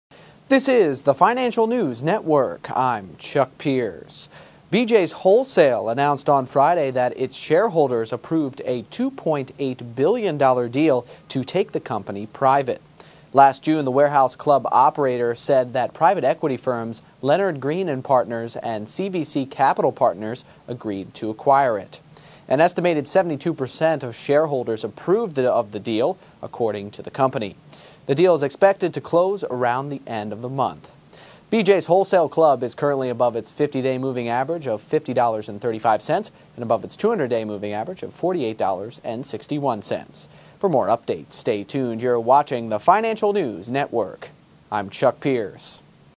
Shareholders of BJ's Wholesale Club Approve Acquisition
BJ's Wholesale (NYSE:BJ) announced on Friday that its shareholders approved a $2.8 billion deal to take the company private.

Last June, the warehouse club operator said that private equity firms Leonard Green & Partners and CVC Capital Partners agreed to acquire it.

An estimated 72% of shareholders approved the deal, according to the company.

The deal is expected to close around the end of the month.

BJ's Wholesale Club is currently above its 50-day moving average (MA) of $50.35 and above its 200-day of $48.61.